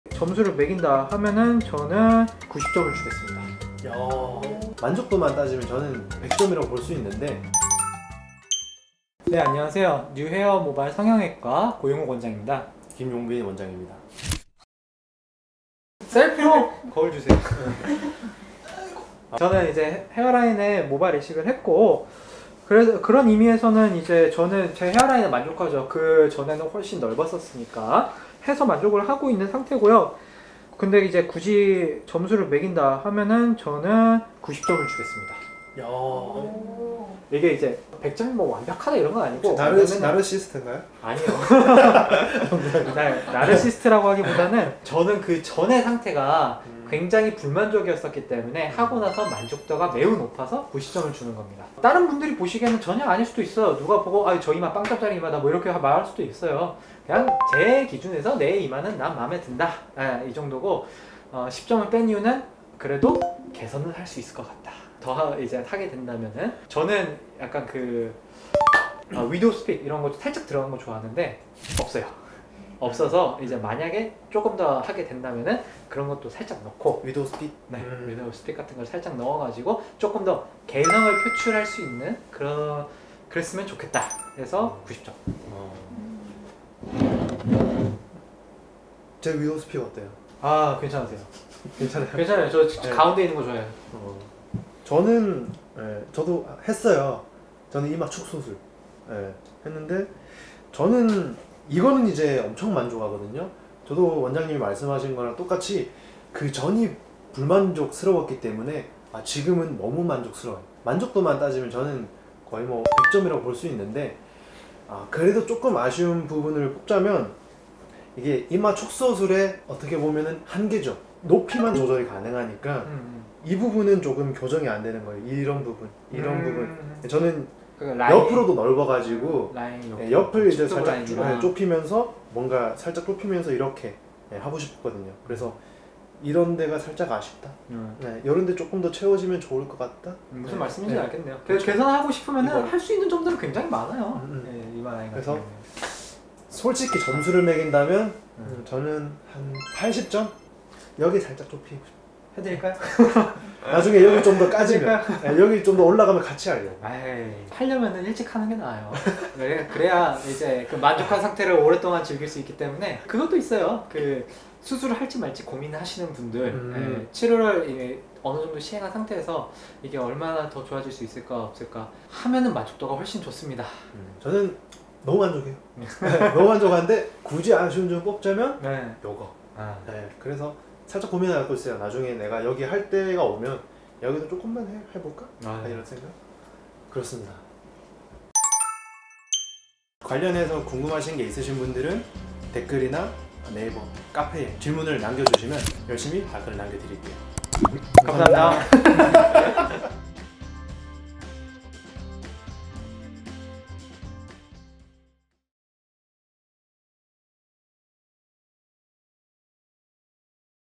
모발이식 후기 만족도 100% 증명합니다 하지만...ㅣ뉴헤어
모발이식과 이마축소술을 한 탈모 의사들의 이야기입니다.
과연 닥터들의 만족도는!?

📁모발이식 헤어라인 교정 영상 보러가기